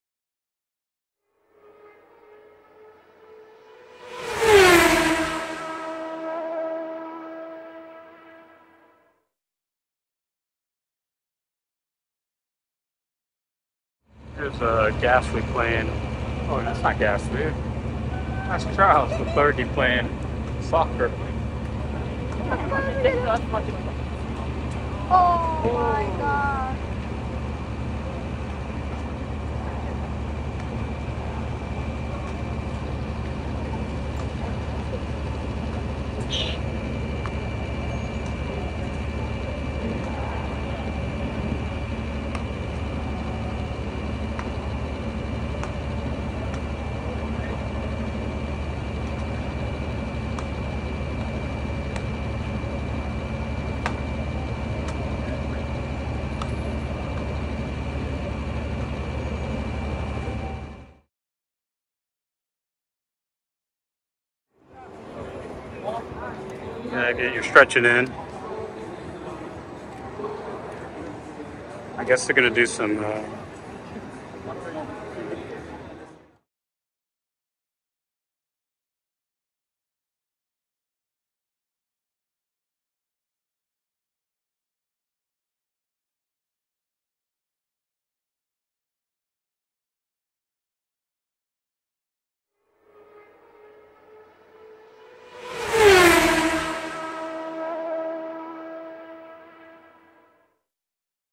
Charles LeClerc playing Football (Soccer)
Charles Leclerc playing football in the paddock.  Ferrari crew stretching before pit stop practice.